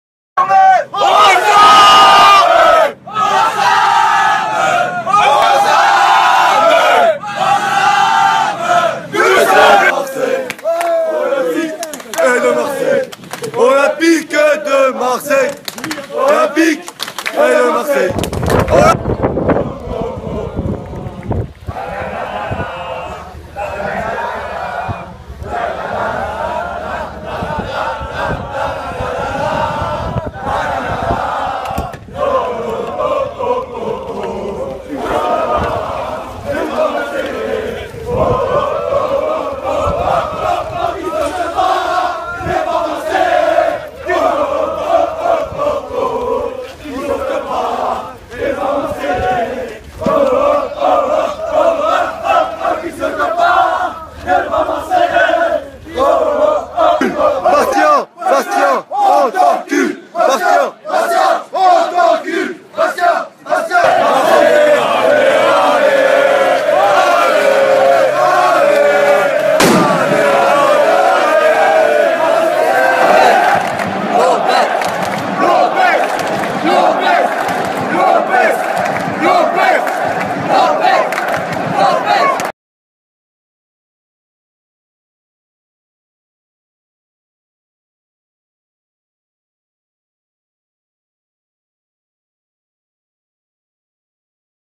Dep sections 21 OM-Bastia
Dernier Match de la saison la Section supporters Marseillais 21 on mis le feu 🔥